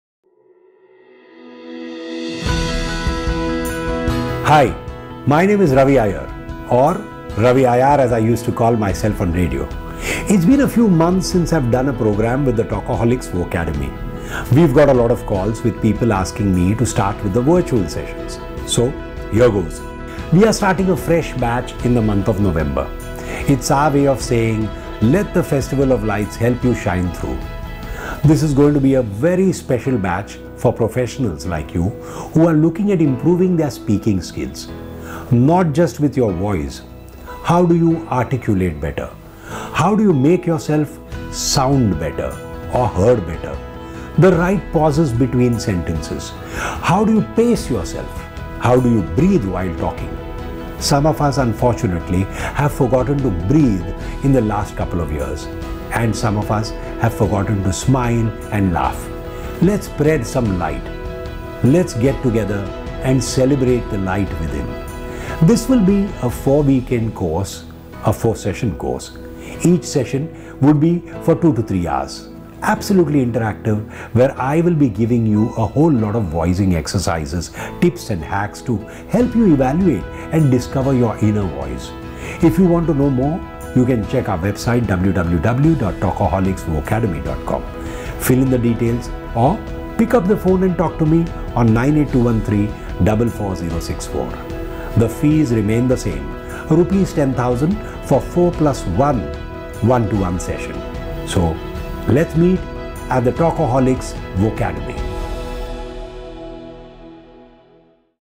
Talkaholics Vocademy is Back With New Batch to Improve Your Speaking Skills.
@talkaholicsproductions  a new batch for the Talkaholics Vacademy starts in November